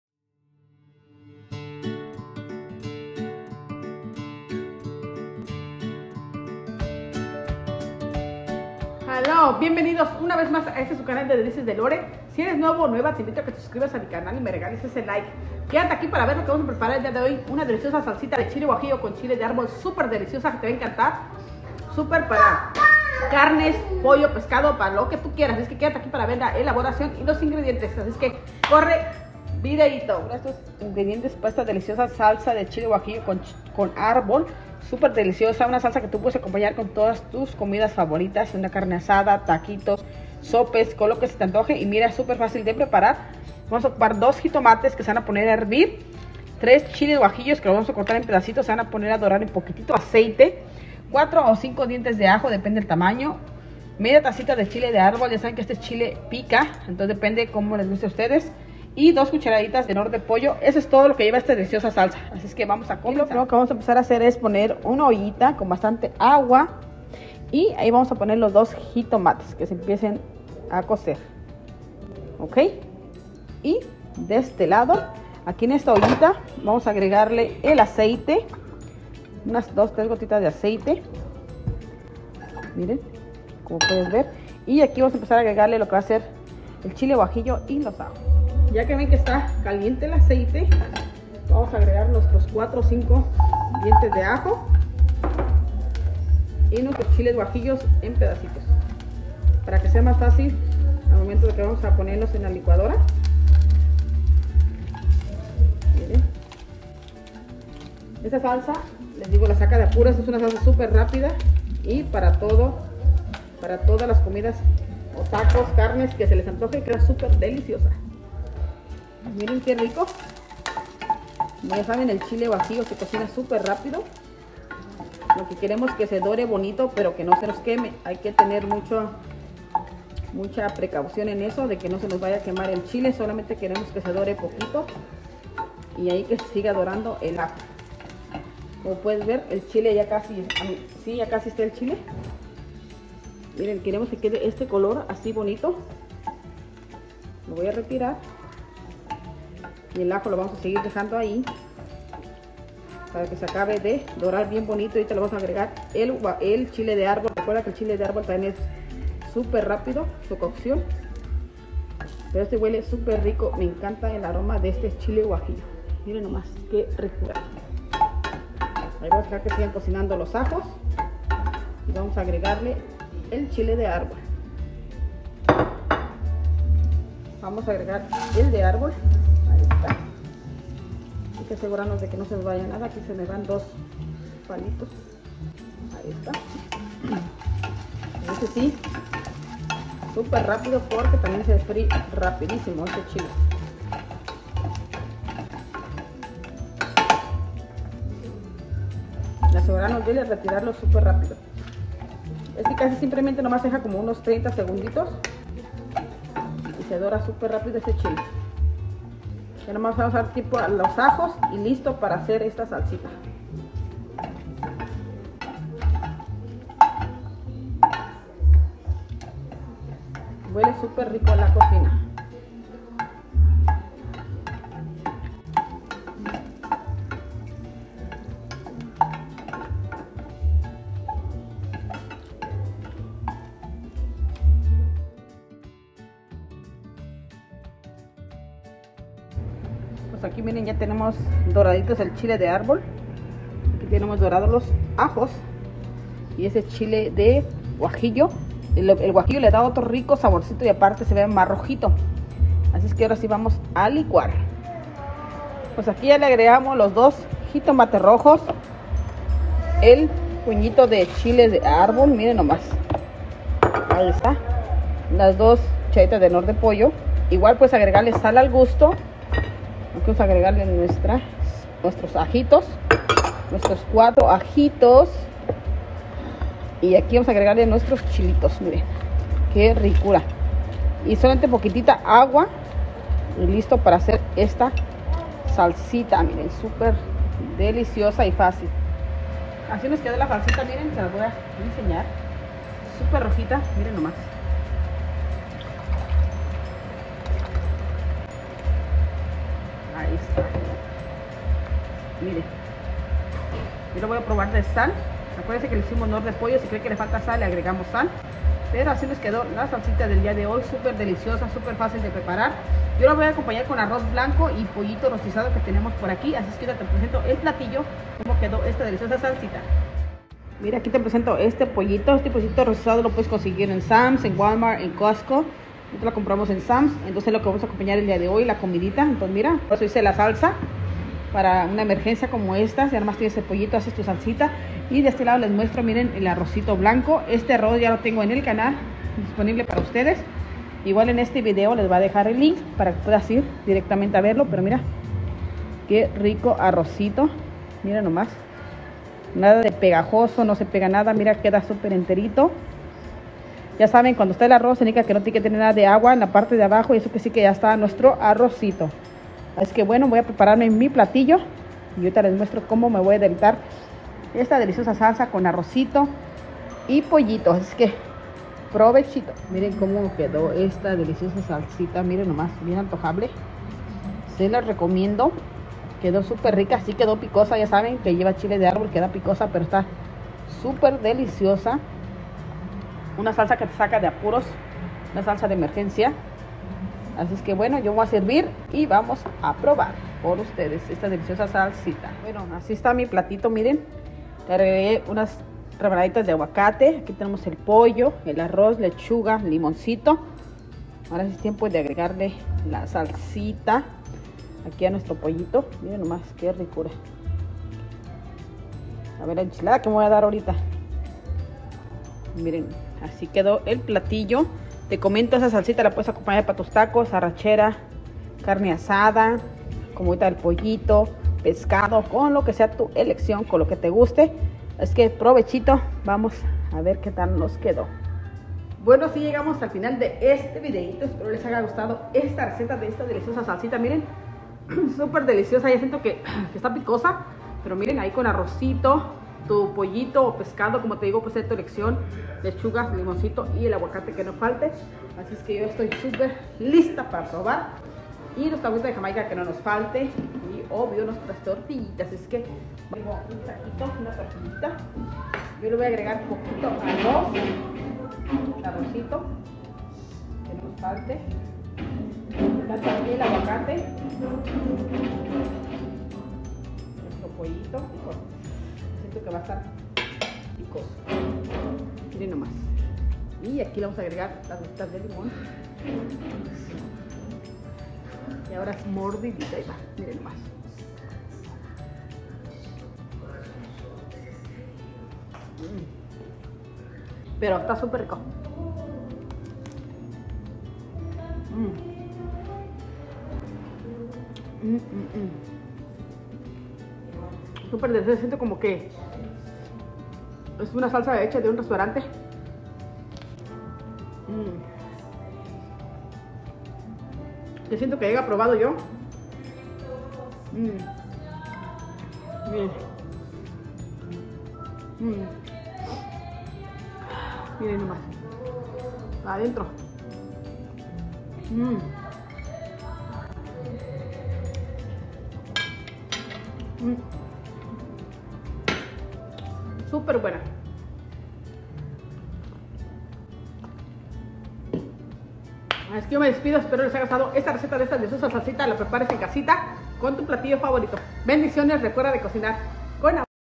Salsa de Emergencia (Chile de Árbol y Guajillo) Deliciosa😜
Muchas gracias por estar aquí apoyándome en este su canal de Delicias de Lore 😍

Ingredientes para esta receta son :

3 Chiles guajillos
2 jitomates 🍅
2 cucharaditas de knorr de pollo
4 dientes de Ajo
1 Taza de Chiles de Árbol
Aceite

Muchas gracias por dejarme entrar a sus hogares muchas bendiciones 🥰🙏👍